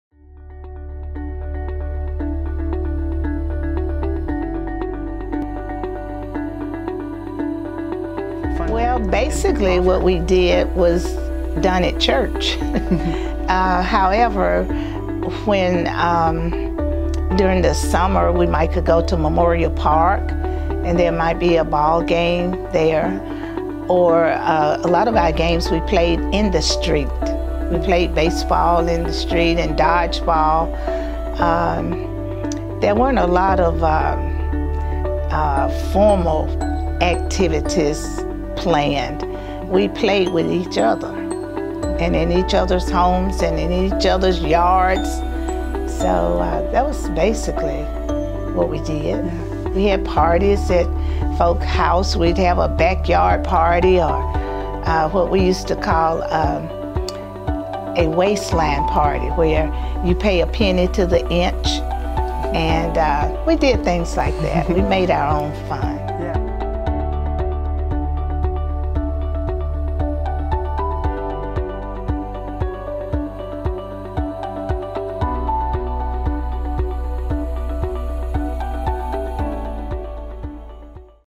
Oral History Question: What did you do with friends after school and on the weekends?
Janice Wesley Kelsey talks about her experience growing up in the North Titusville community!

"What did you do with friends after school and on the weekends?"

Check out this snippet of an Oral History project we're doing with members of the North Titusville community!